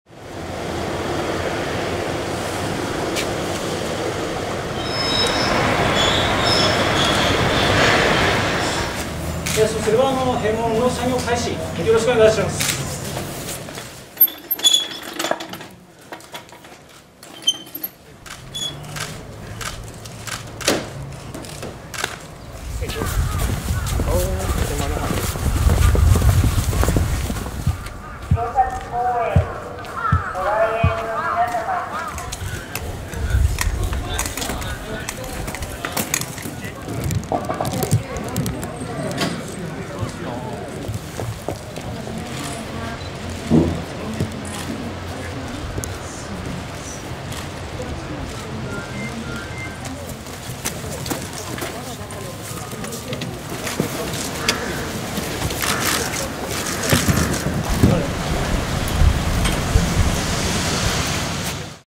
弘前公園初の完全封鎖 5月6日まで
新型コロナウイルス感染拡大を防ぐため、弘前市の弘前公園が10日、閉鎖された。